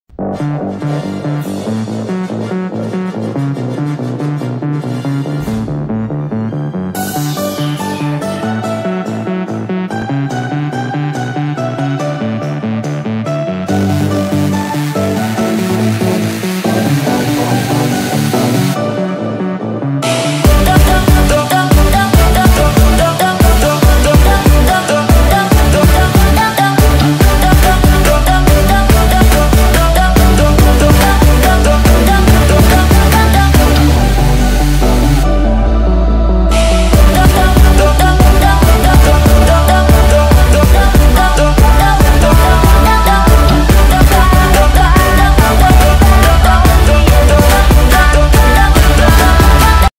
all play buttons that YouTubers have
hi guys hmmm this play buttons are real and subscribe to those I used their play button on this yt vid subscribe to me so as they have a beautiful day bye!